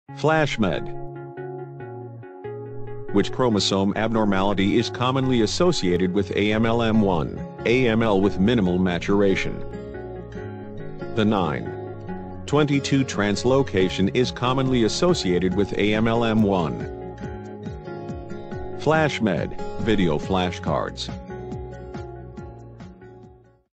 t(9;22) chromosomal abnormality AML
Which chromosome abnormality is commonly associated with AML M1 (AML with minimal maturation)?
 Answer: t(9;22)